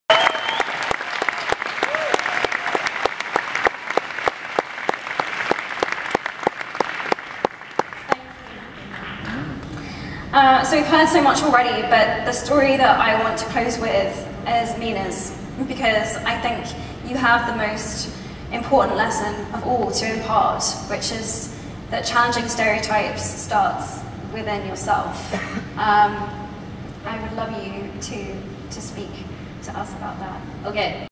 One Young World Summit 2016 - Emma Watson speaking to Mina Tolu
Emma Watson speaking to Mina Tolu (communications officer at Transgender Europe) at the One Young World Summit 2016 in Ottawa, Canada